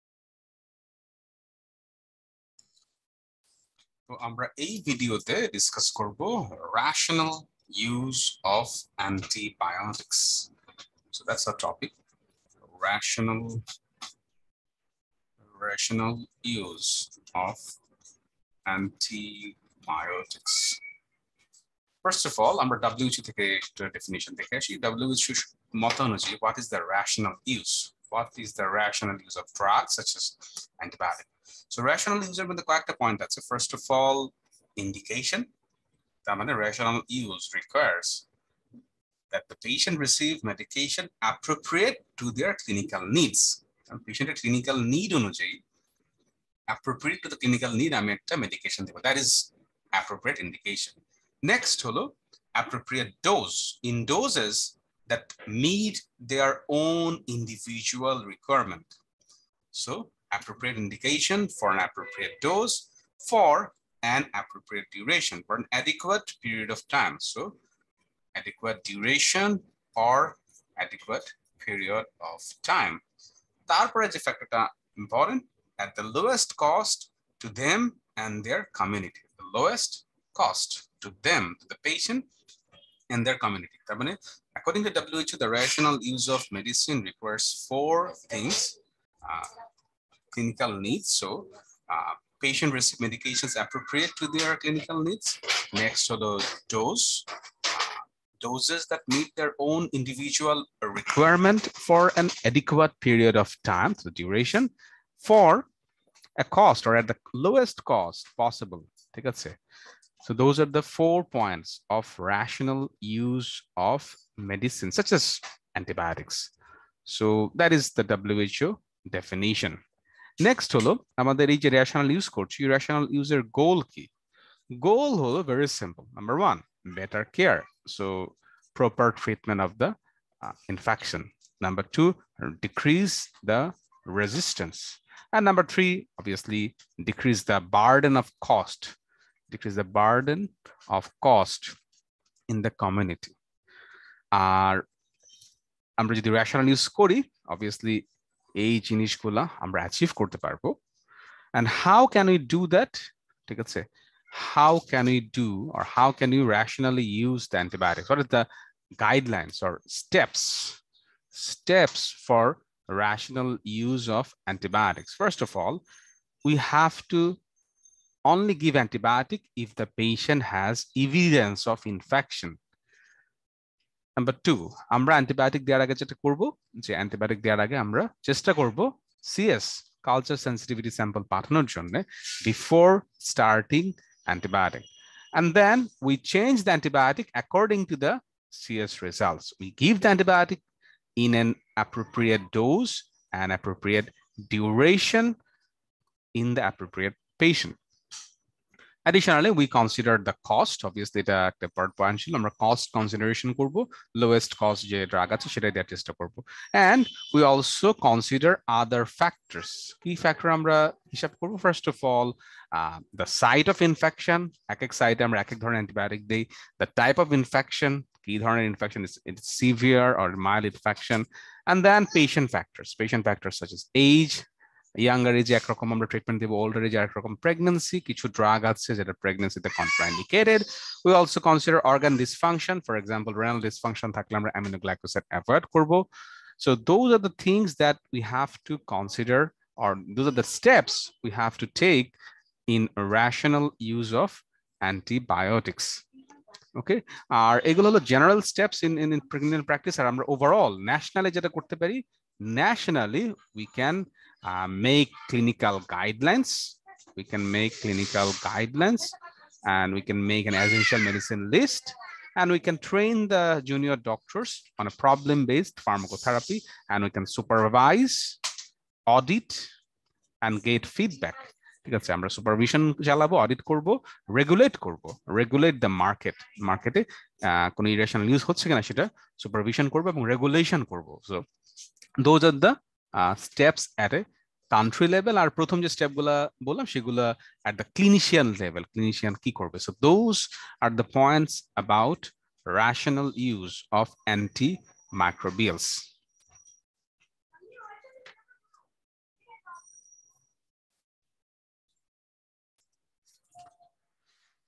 Rational use of antimicrobials (according to WHO)
This is a high yield summary of what it means by rational use of antimicrobials, goals of rational use, and steps to consider at a clinical practice and national levels